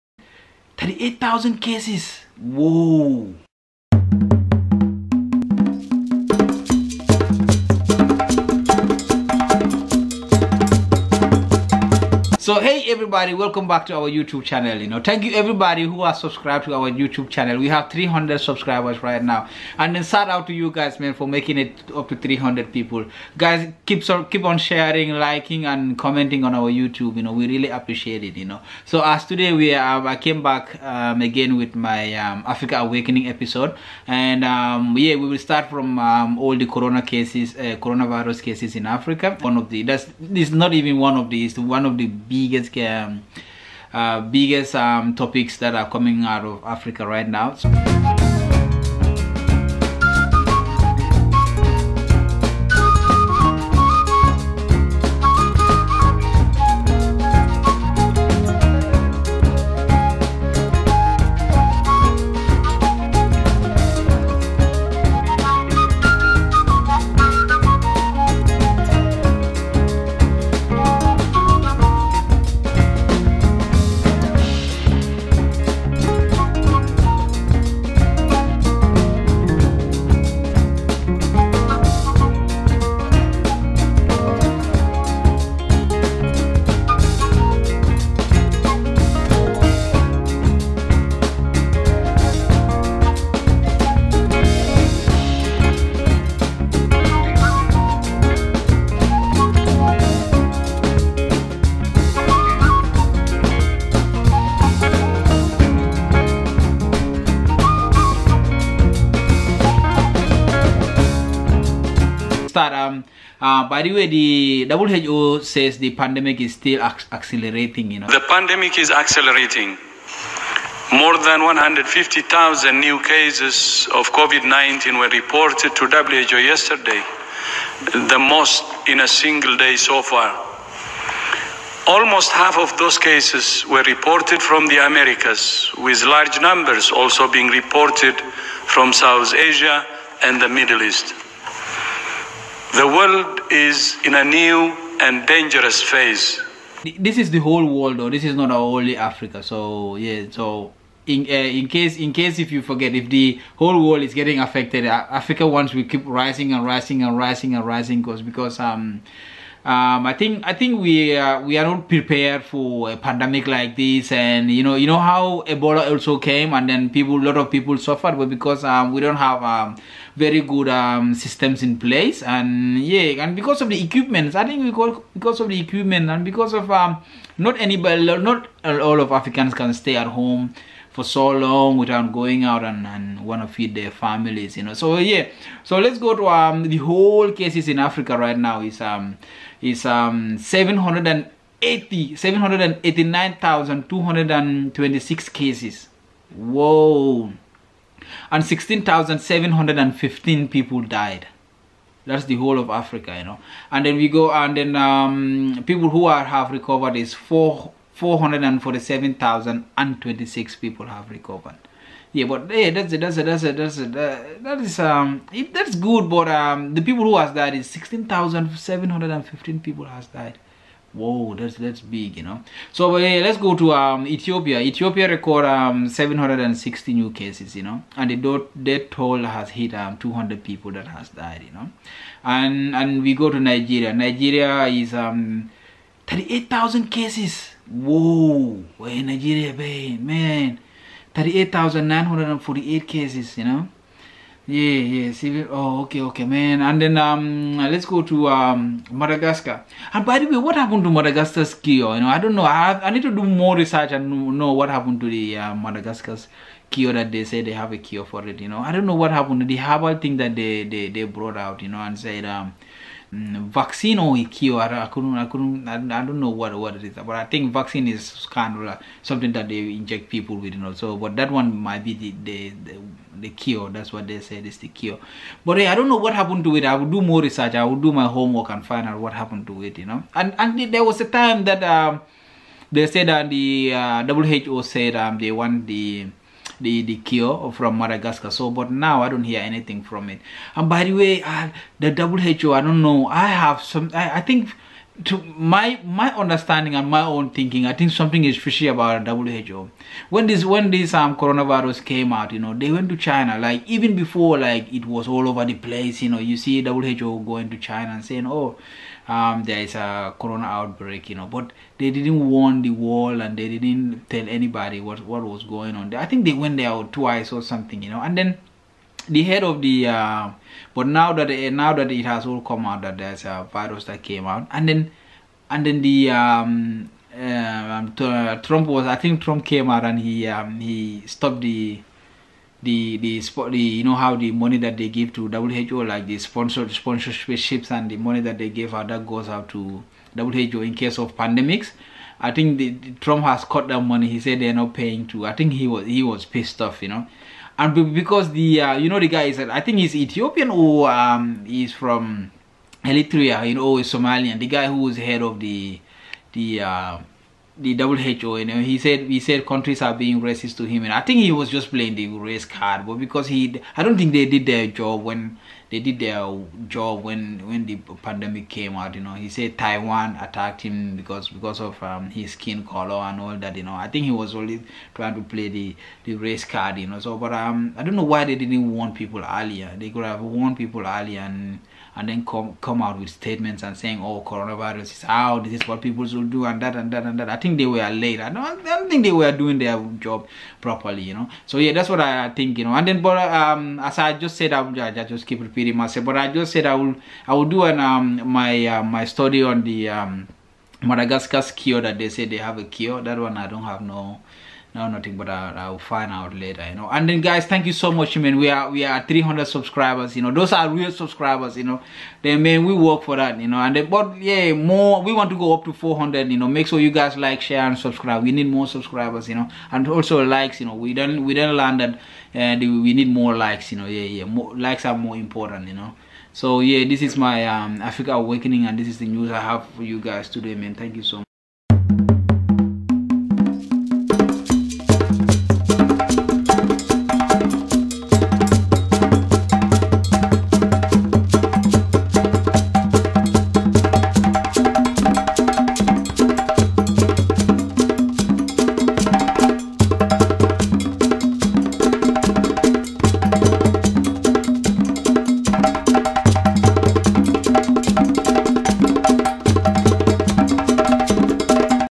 CORONA-19 IS ACCELERATING - CORONA-VIRUS IN AFRICA
Nuha talks about corona and how it is affecting Africa.